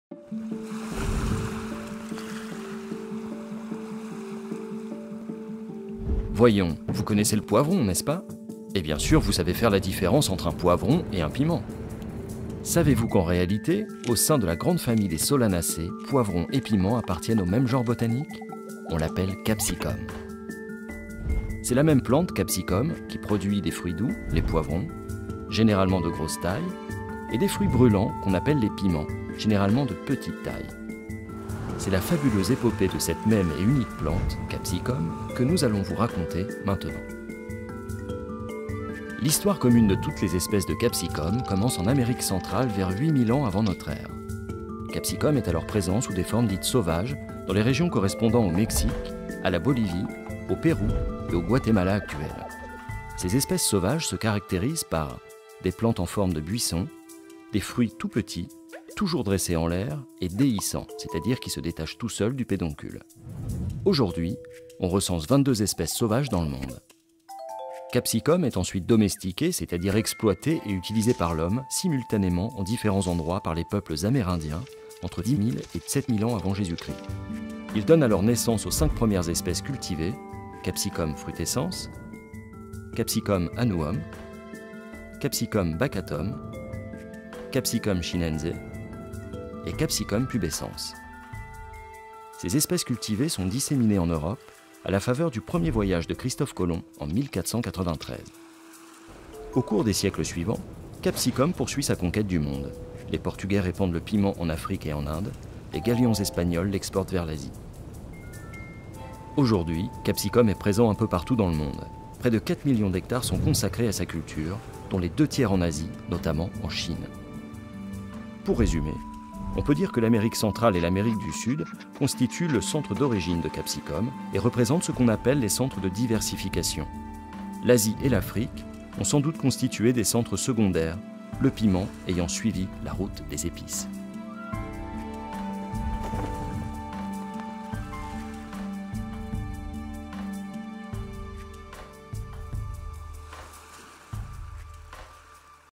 The fabulous story of Capsicum
The story of peppers and chillies (Capsicum), from 10.000 years ago to nowadays.
Wild species, domesticated species, centre of origin, centres of diversification, secundary centres...
An educational video clip designed by CultiVar and Artcam Production.
Scientific expert: Anne-Marie Sage Palloix / INRA.
2017